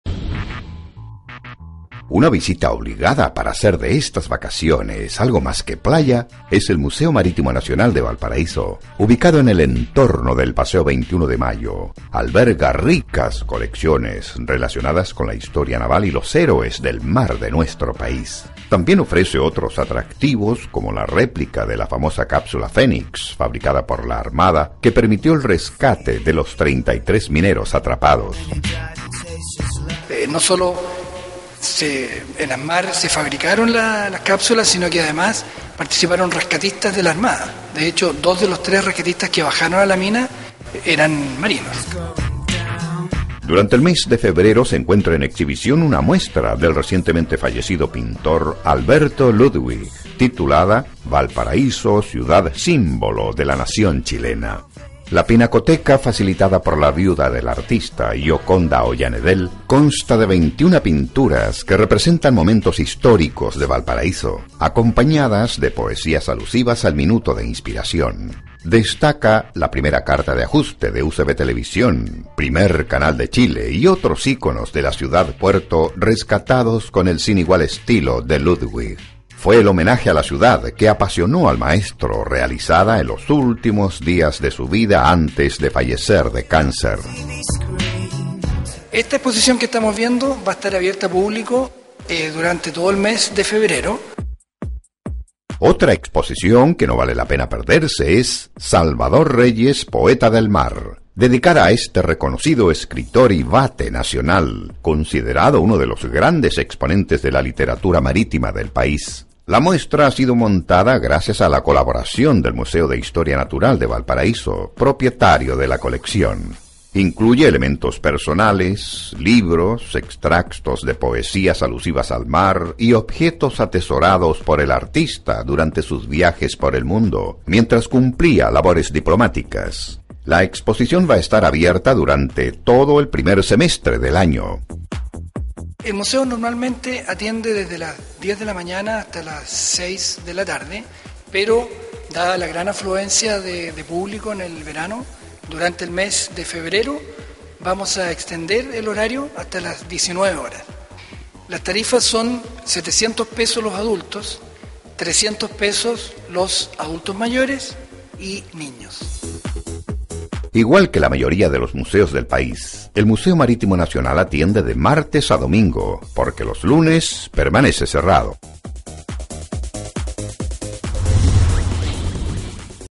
Museo Marítimo.flv
Selectas colecciones ofrece este verano el Museo Marítimo Nacional ubicado en el paseo 21 de Mayo en Valparaíso. Una de las exposiciones se refiere al poeta y escritor del mar Salvador Reyes. Otra versa sobre el pintor Alberto Ludwig y las obras que dedicó a la ciudad patrimonial.